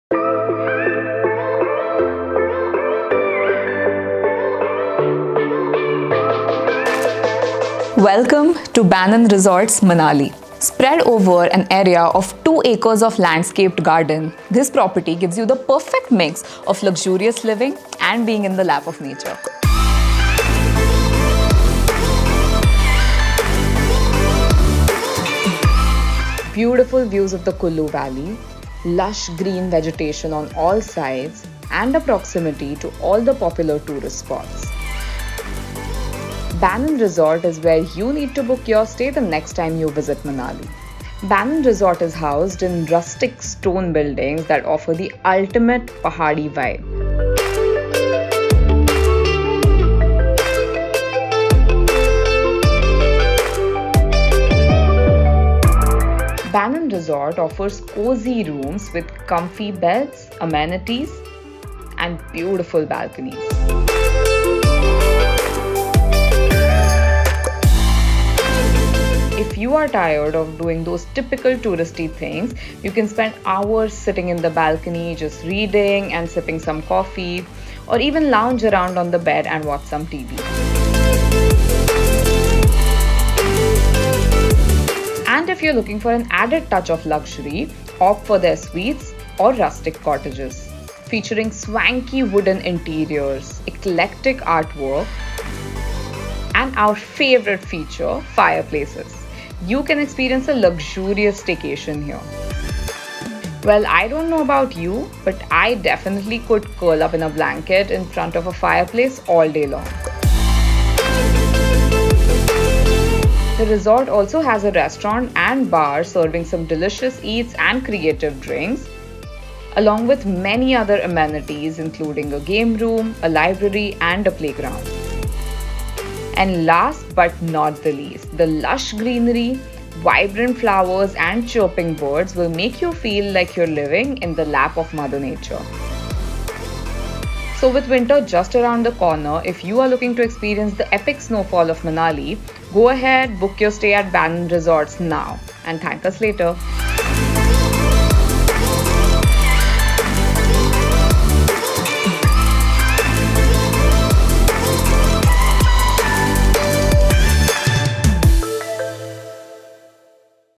Manali's Most Beautiful and Luxurious 100 Years Resort | Banon Resorts
When we’re seeking abreak from the stresses of city life, nothing can beat the fresh air and pleasant climes of Manali. Add to that the added beauty of the winter snow, and this popular getaway destination is made that much more inviting. So, we’re here to tell you about Banon Resorts, which is tucked away in the hills of Manali, and will make you want to pack your bags and head off on a trip right away!

Offering a picturesque view of the mountains and lush greenery all around, Banon Resorts is located on Old Manali Road and is the only resort with cottages in Manali. Sprawled over two acres of landscaped gardens, the resort was built and is managed by the descendants of Capt A.T. Banon, who is said to have started the first hotel in Manali in the early 20th century.

With almost 100 years of experience in the hospitality industry, these folks provide their guests with an experience to remember by combining the past and the present. This way, you get to enjoy the ancient cultural heritage of Kullu Valley in a modern setting with all the comforts of the world! As you walk around the resort, you’ll notice a beautiful amalgamation of traditional architecture and modern designs here that blends beautifully into the environment to give you the best of both worlds.

What we loved the most about this luxurious resort was the picturesque gardens where you can enjoy a picnic with your family, or sit in solitude to enjoy the nuances of nature. The resort has 32 cosy rooms, three huge cottages, and three in-house restaurants where you can enjoy some of the best food in Manali.

All in all, if you’re planning a trip to Manali soon, or have been prompted by this video, then this hotel should be at the top of your list. Grab your squad and head off right away!

for more details.